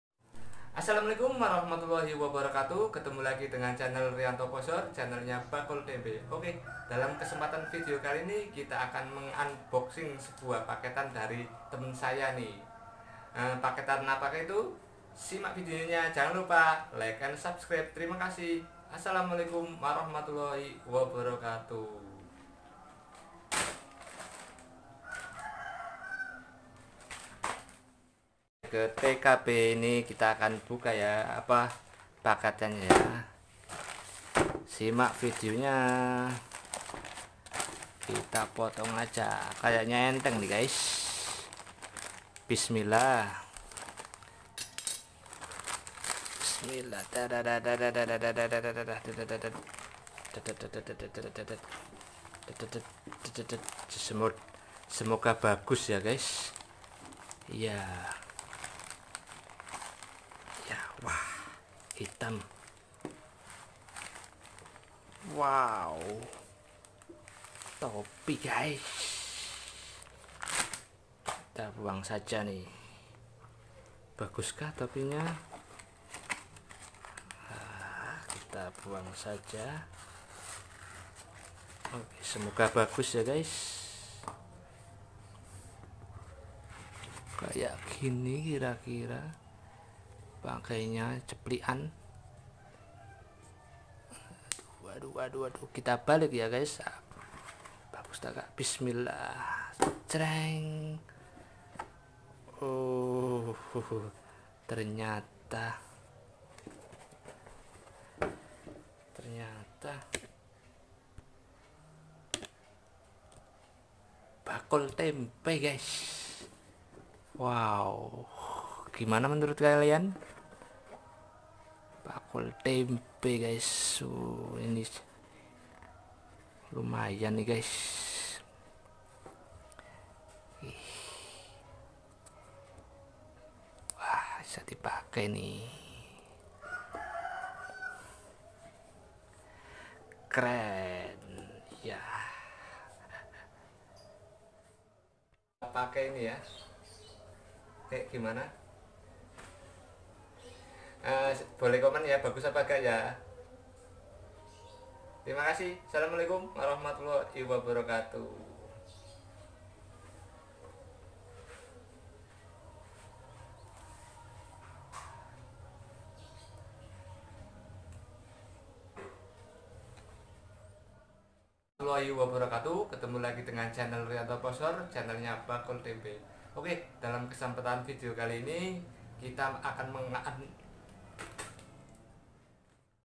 Topi bakul tempe bermakna
Topi bakul tempe bermakna dapat kiriman paket dari teman bermakna karena ada tulisan bakul tempenya